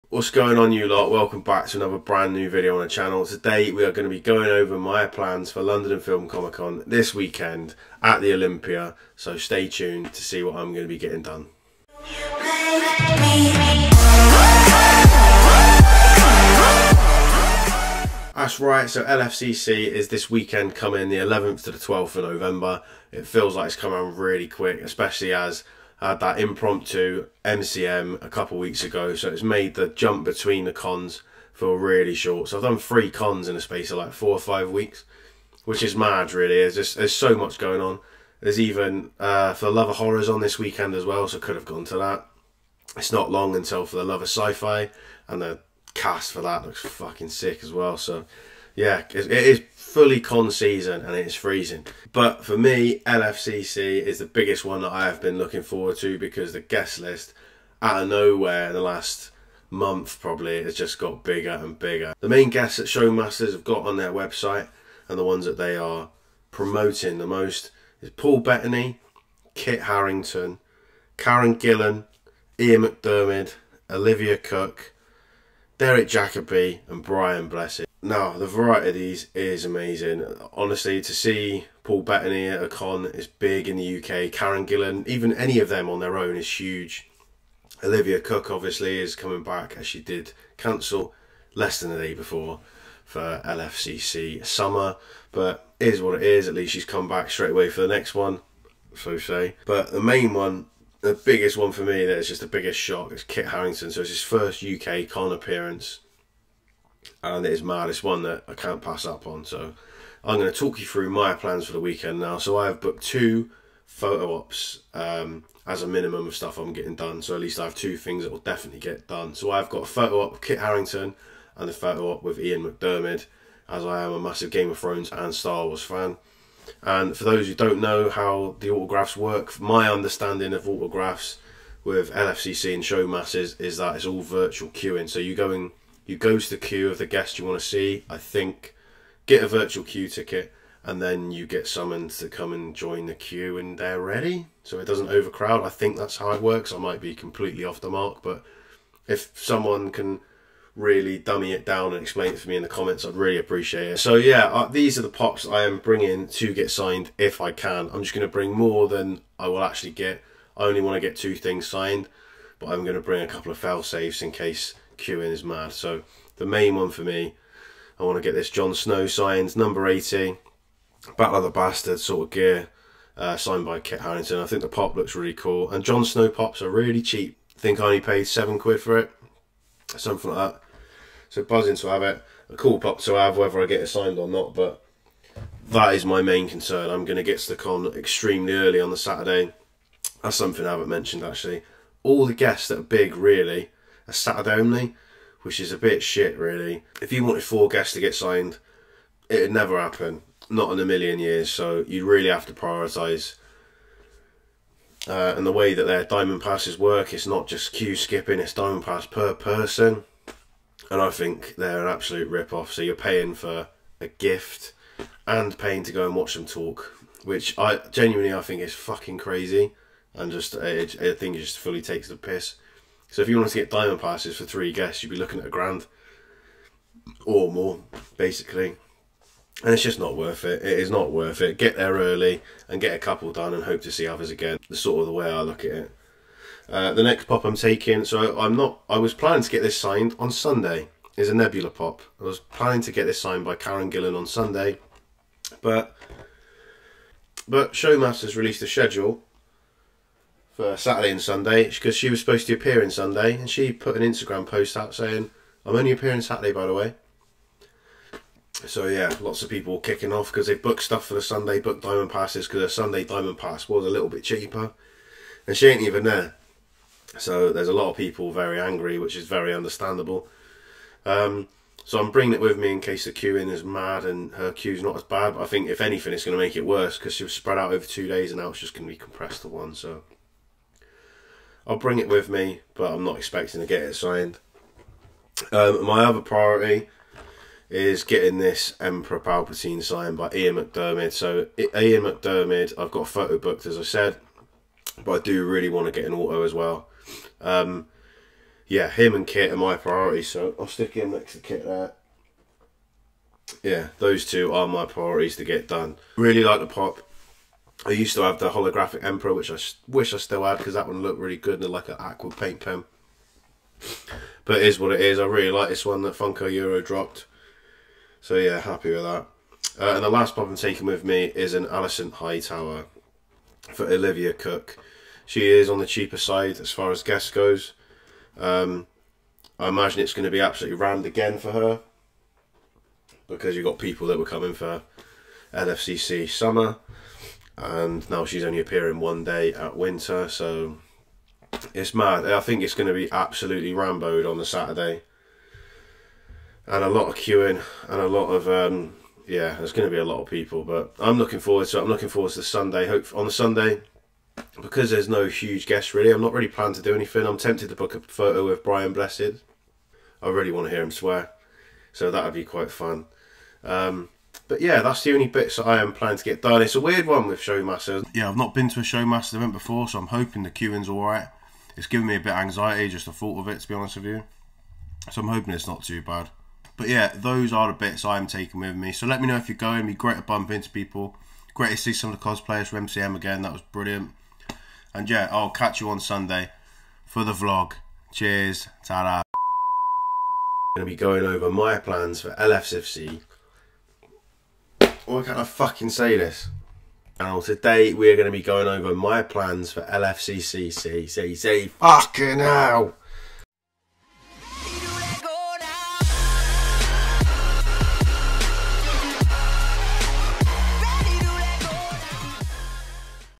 Preparing for London and Film Comic Con Winter 2023!
Hello There! In today's video I show you my plans for London and Film Comic Con Winter 2023 this weekend at Olympia in London! Are you going to the Con this weekend? Let me know who you are planning to meet and what your plans are in the comment section below!

And remember, the force will be with you, always.

Dino Joe